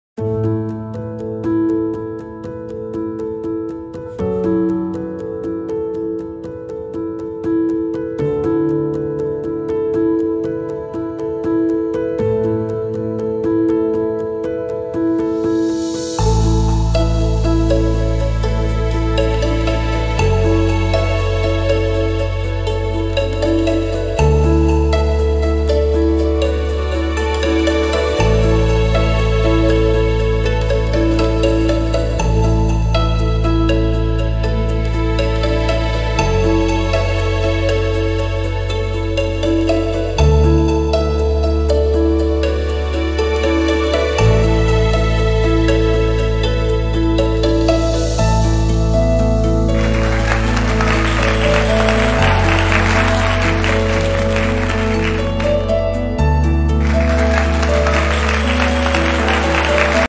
Telugu Activity "హస్యకథ", 3వ తరగతి/మర్యాద చేద్దాం. మా సంసిధ్ శ్రీలక్ష్మీ విద్యార్థుల కథా సేకరణ,KNL.
తెలుగు కృత్యం, హాస్యకథ, పరమానందయ్య శిష్యుల కథ.